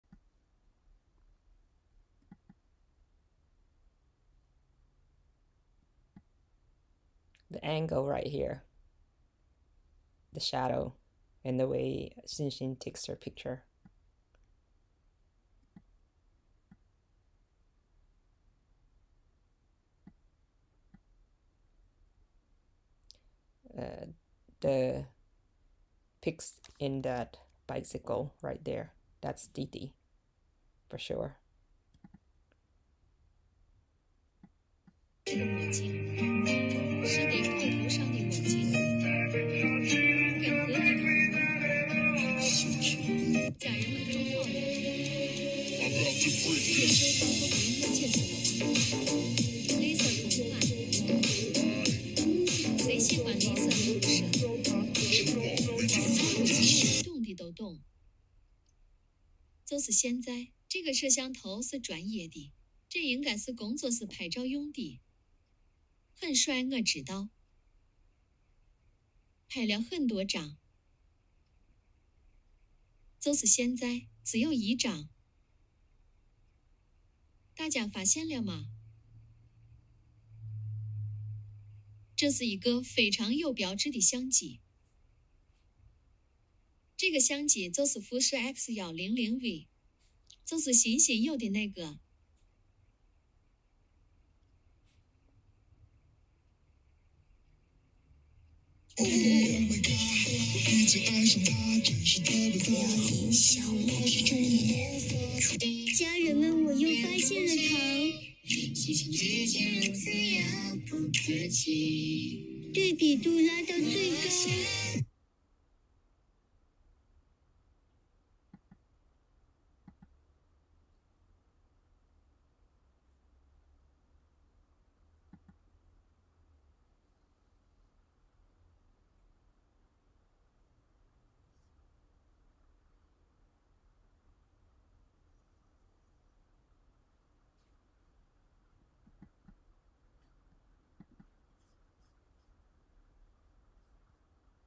dog1 Dylan Wang & Esther Yu's dog "Koki"
about a month ago, there was a big fuss about Koki's the dog  social media account  whether the dog belogs to Esther's friend dog or DD & XX's dog..  I was watching the whole story unfold in front of my eyes, those netizens in china works so fast and they are able solve the mystery.

We will see how important this dog plays a role in solving the puzzle.